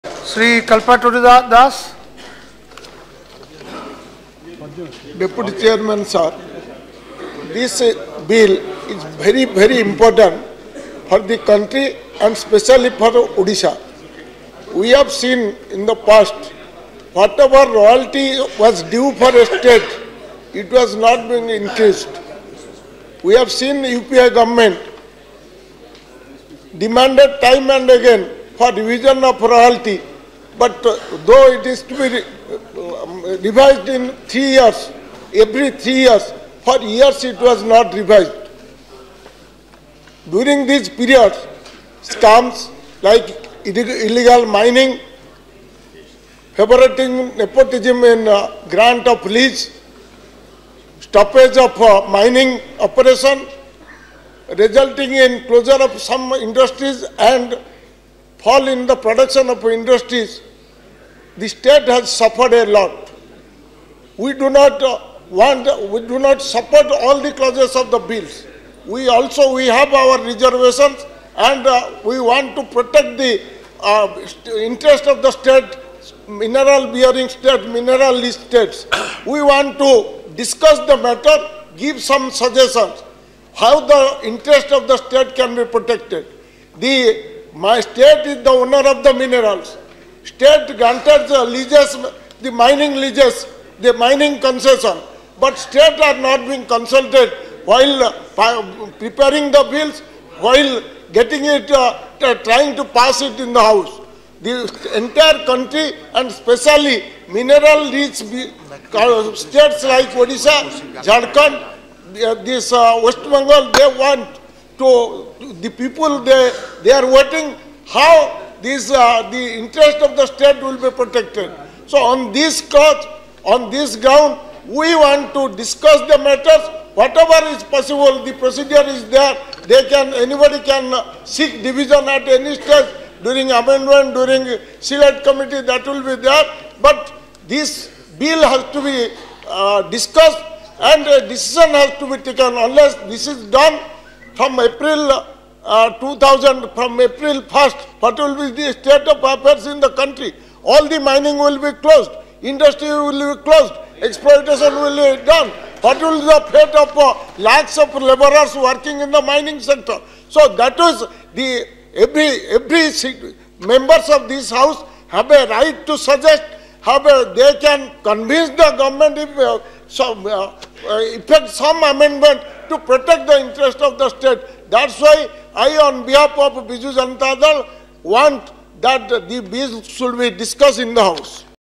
Sh. Kalpataru Das’s comments on The Mines and Minerals Amnd. Bill, 2015
Discussion on The Mines and Minerals (Development and Regulation) Amendment Bill, 2015.

March 10, 2015 | Rajya Sabha Budget Session - 234